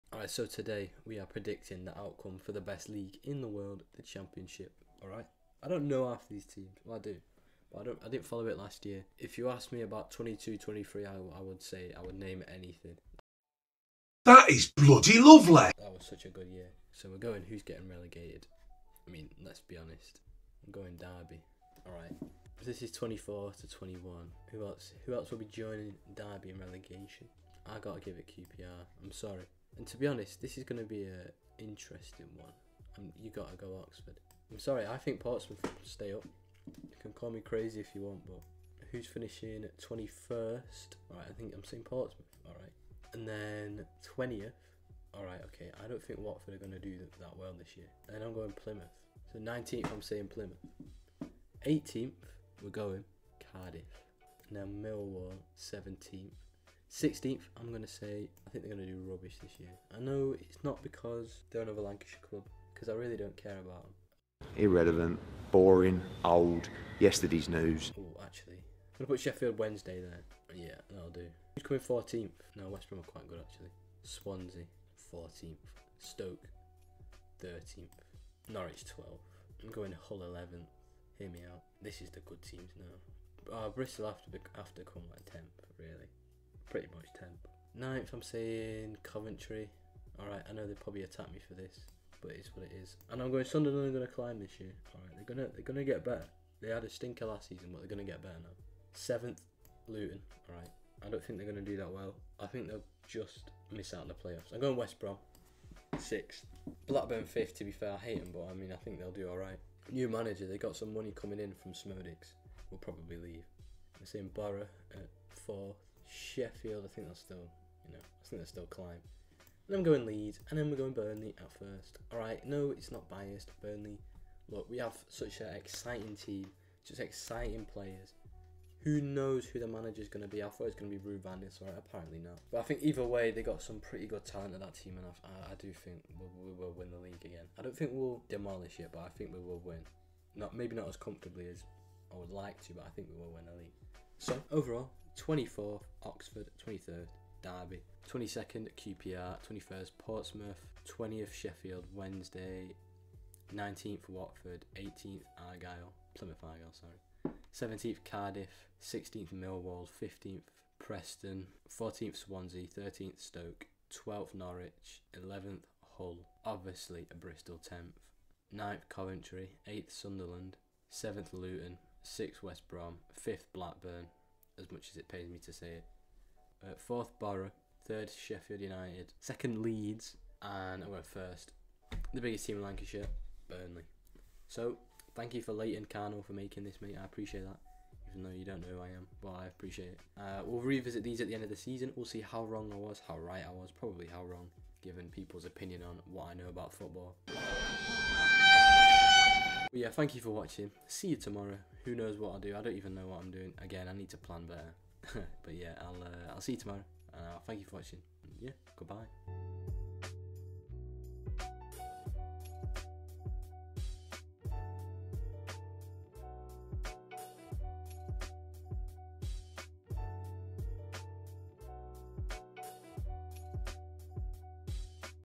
MY 2024/2025 CHAMPIONSHIP PREDICTIONS!
In this video i predict the outcome of next seasons championship season.